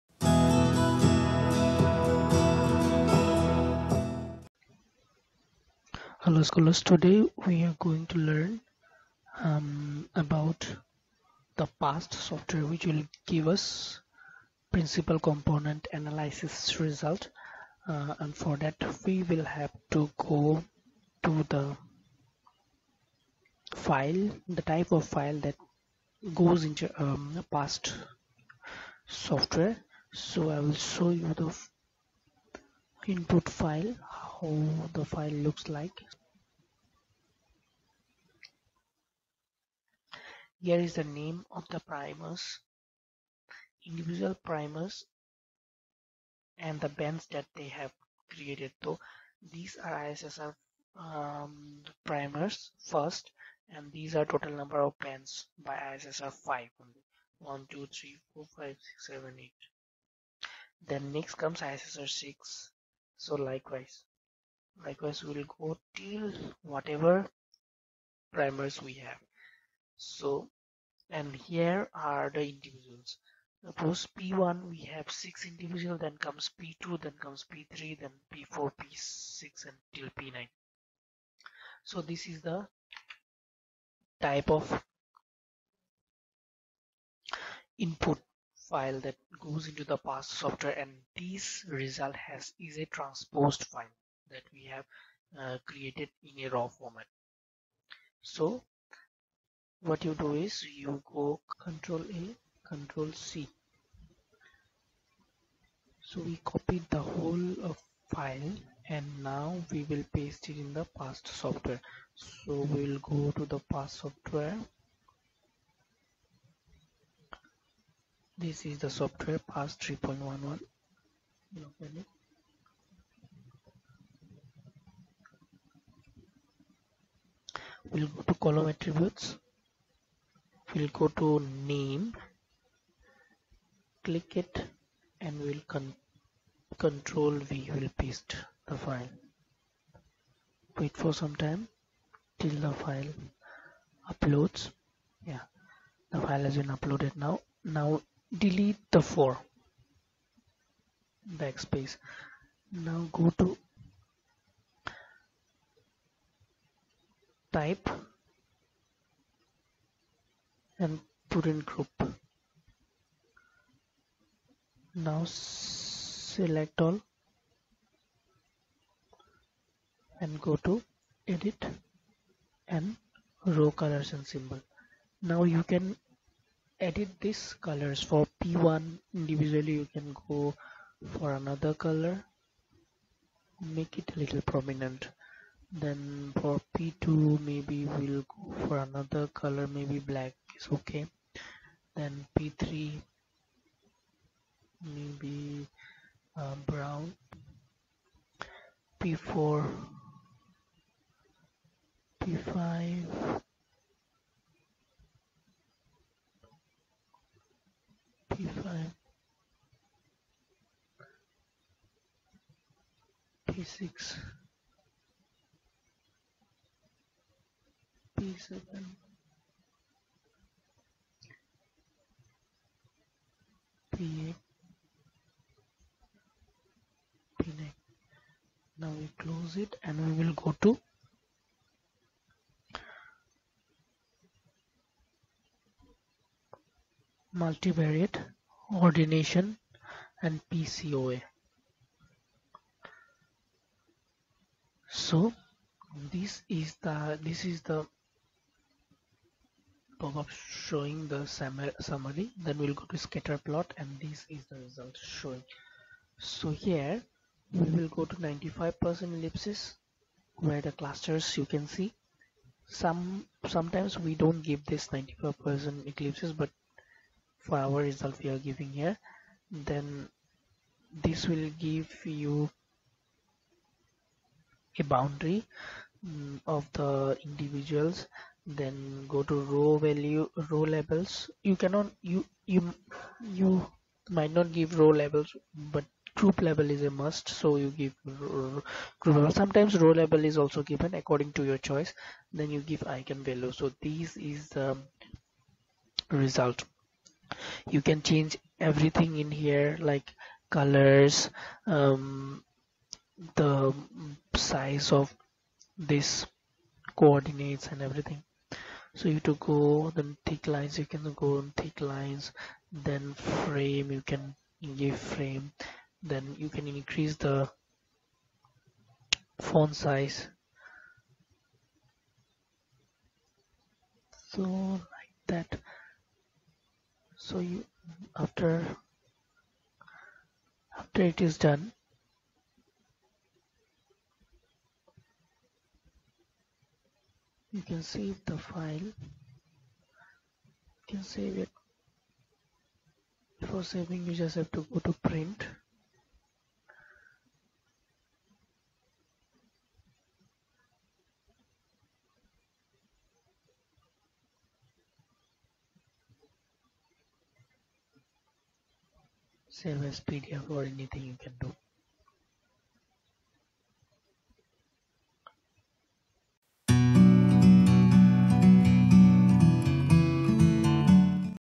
PAST tutorial (PCoA)
Viewers can learn to plot PCR marker based result to find out the clustering pattern of their population as per their DNA variability. This is merely a simple way to predict the number of clusters that the banding profile of your population forms. Give us an in-depth knowledge which individuals are closely or distantly related based on the principal component analysis result (PCoA).